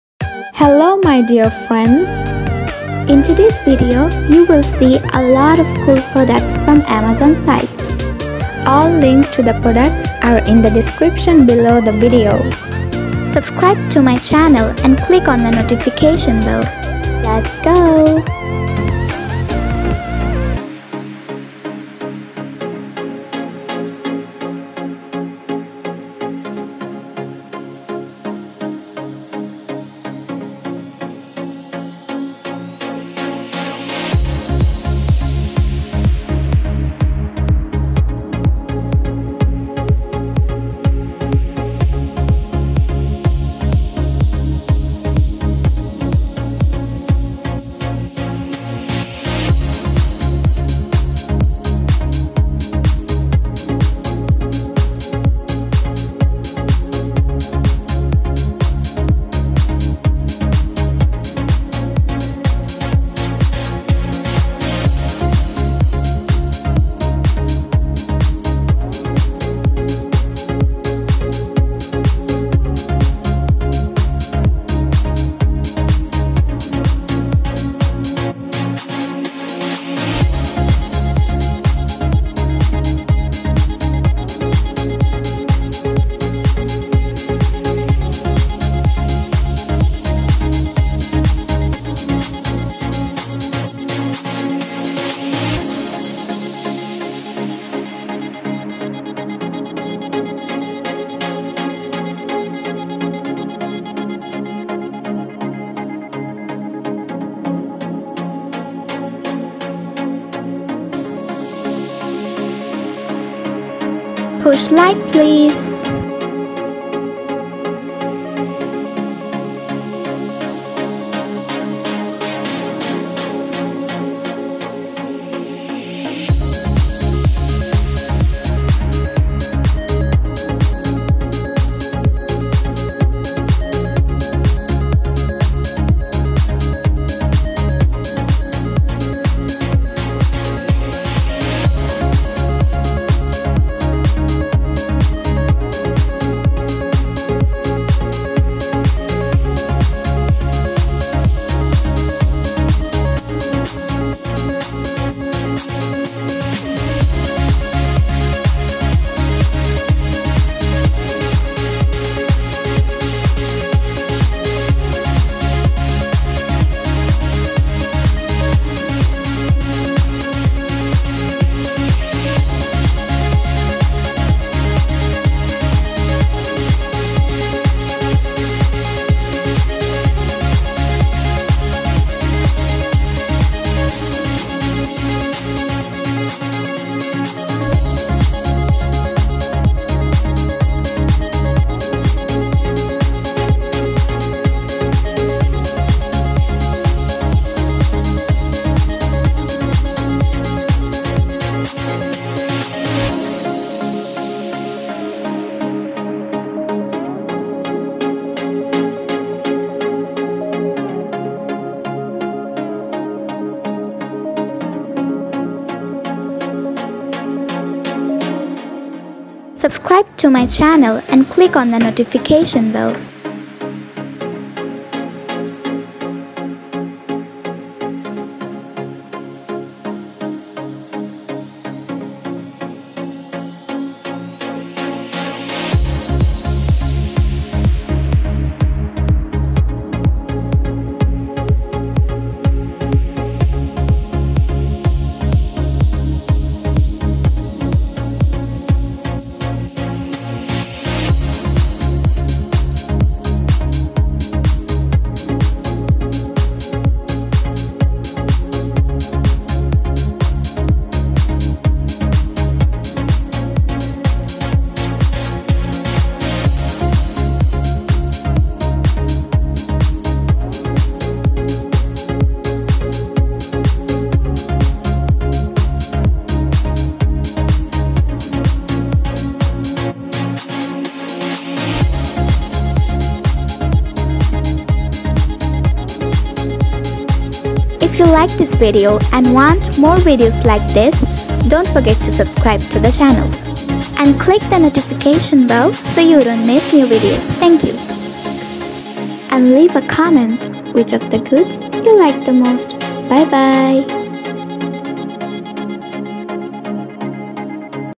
7 Cool And Useful Car Accessories You Must Have In [2021]
In this video I will show you amazing and coolest products from Amazon.
Today you gonna see a lot of cool stuff, that'll improve your safety, cleaning skills and take care of you health.
👇All the links to these essentials can be found In the description below the video👇

2.Car Air Inflatable Travel Mattress Bed

4. Graco 4Ever DLX 4 in 1 Car Seat

5. Emergency Seat Belt Cutter & Window Breaker

6.Push Bumper Fastener Rivet Clips

7. Torq Foam Blaster 6 Foam Wash Gun

It supports my channel and allows me to continue making videos.